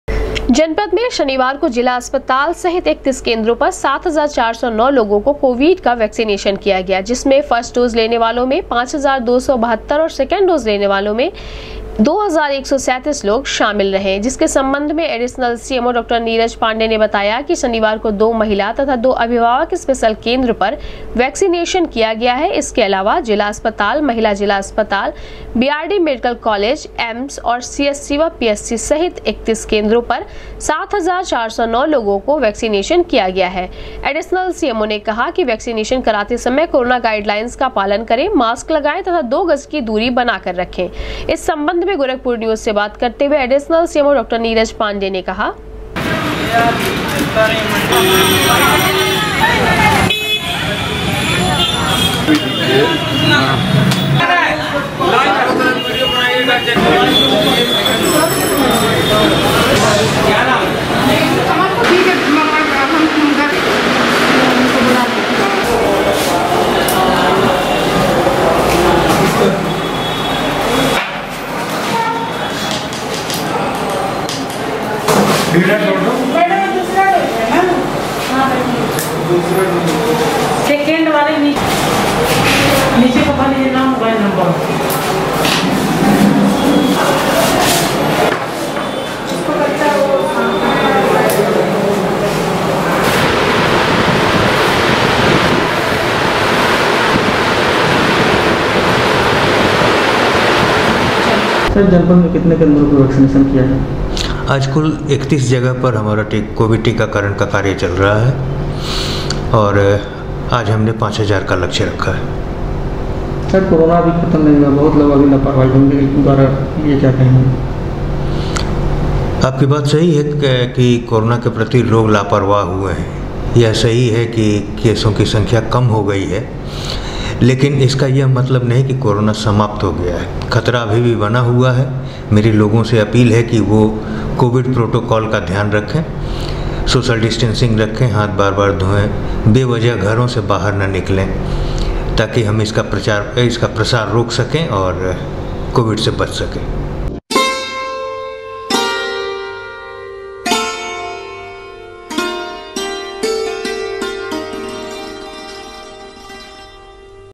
शनिवार को जिला अस्पताल, सहित 31 केंद्रों पर 7,409 लोगो को किया गया कोविड का वैक्सिनेशन, एडिशनल सीएमओ
शनिवार को जिला अस्पताल, सहित 31 केंद्रों पर 7,409 लोगो को किया गया कोविड का वैक्सिनेशन, एडिशनल सीएमओ ने कहा, वैक्सिनेशन कराते समय करें कोरोना प्रोटोकाल का पालन, रहे सुरक्षित